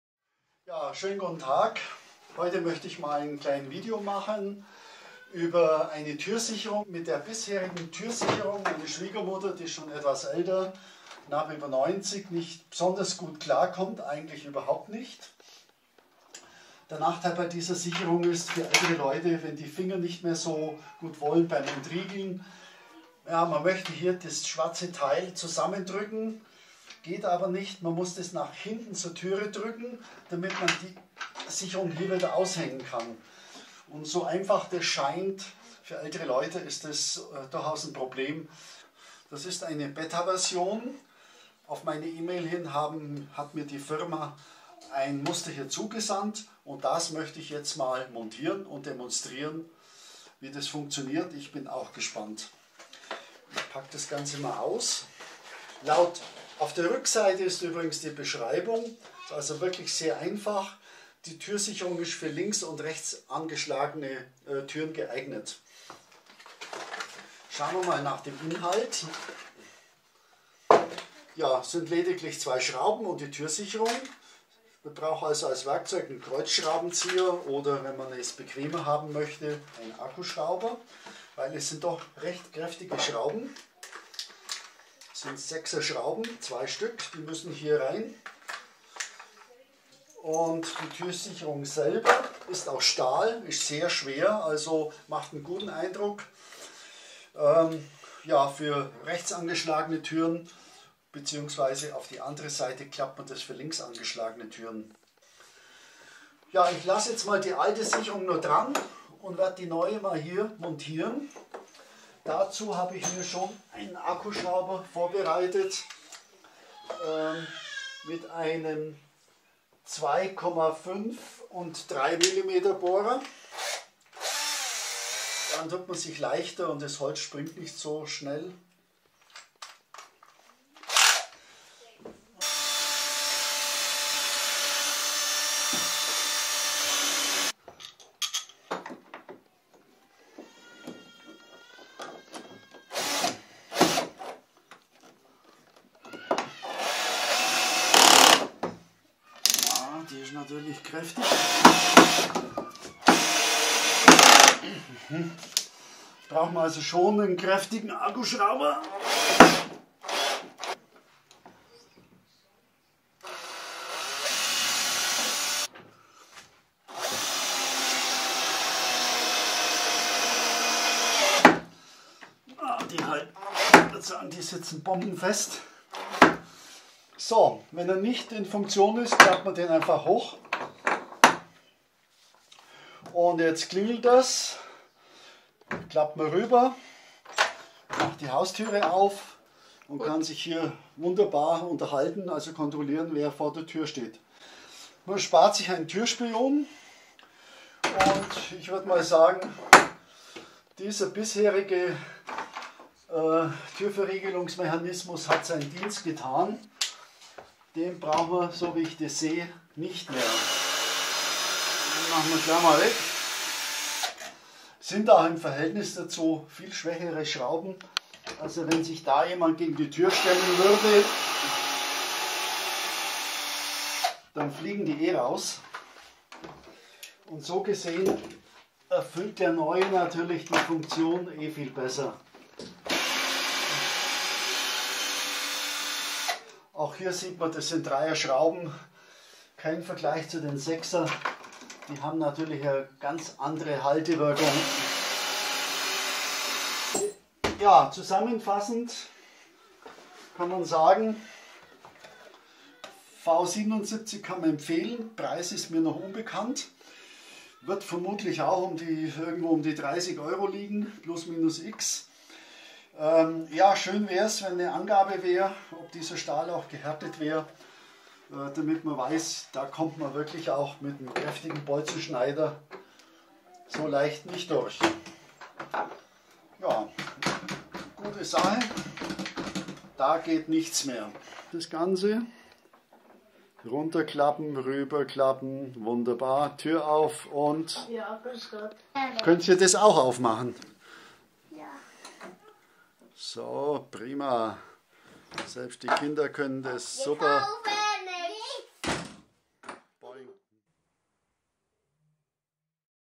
Die effektive Türsicherung. Die stabile Alternative zur Türkette aus massivem Edelstahl.
Entdecken Sie die innovative Alternative zur klassischen Türkette mit unserer stabilen Türsicherung. Öffnen Sie Ihre Haustür nur einen Spalt breit – ideal für Senioren und Kinder! Einfache Montage mit nur zwei Verschraubungen, passend für Rechts- und Linksanschlag. Hergestellt in Deutschland aus 100% hochwertigem Stahl.